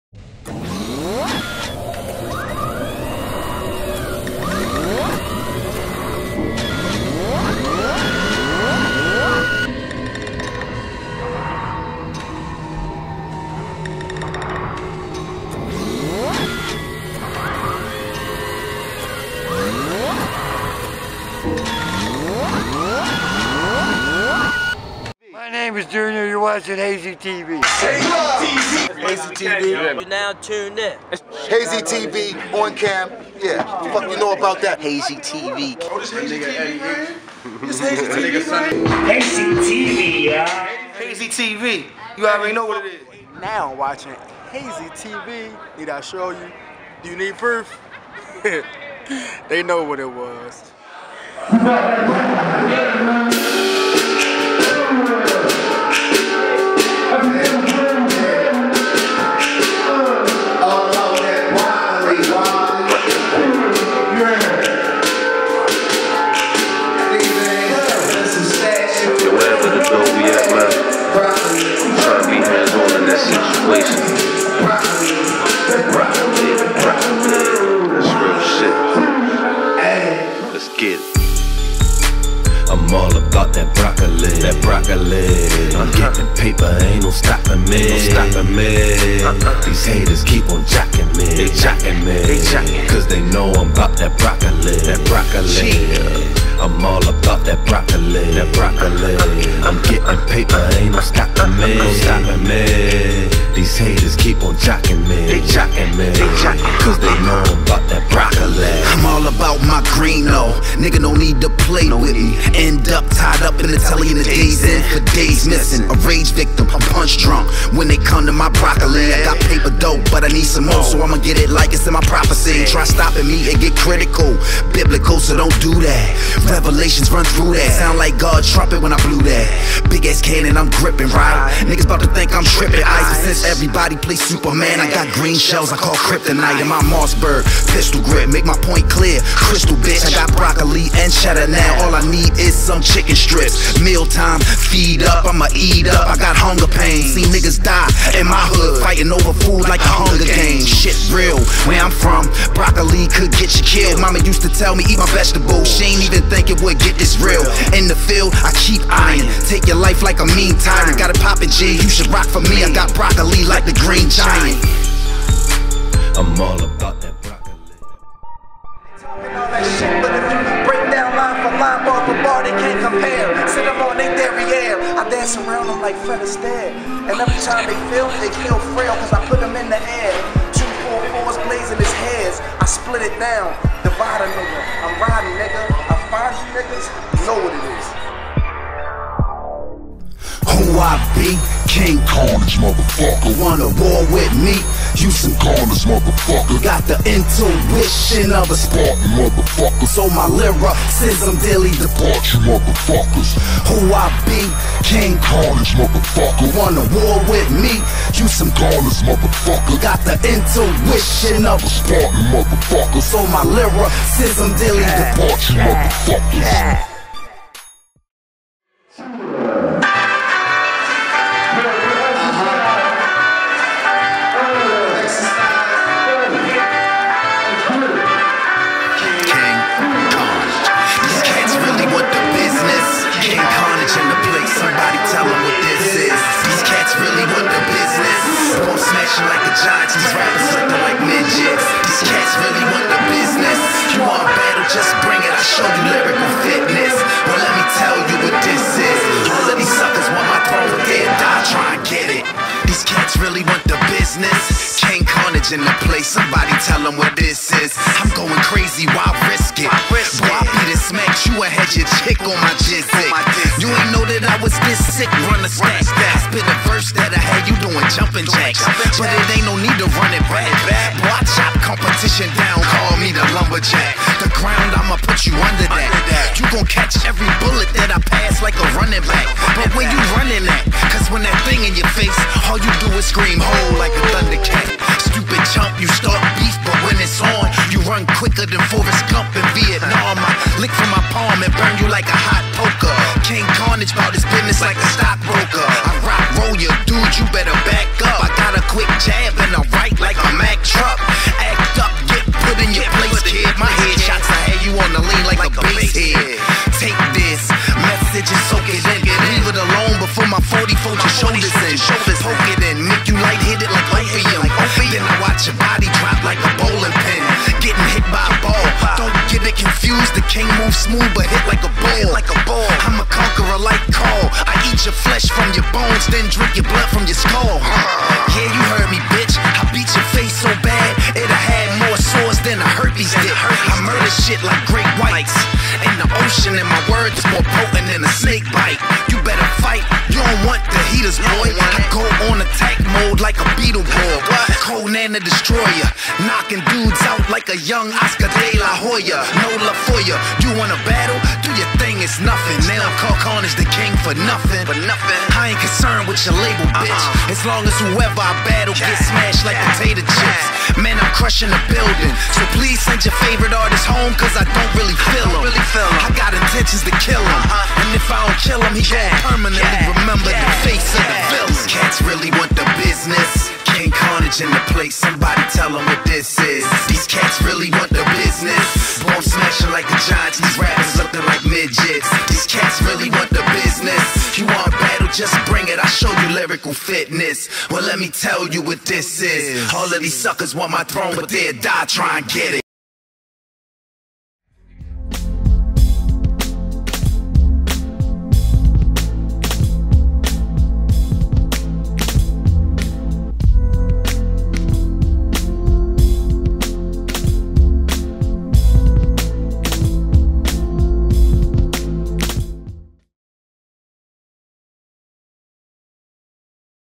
HAZEY T.V. PRESENTZ KING CARNAJE AND HAZEY FAM
CHECK OUT KING CARNAJE GO IN LIVE @PAPARAZZI IN BALTIMORE!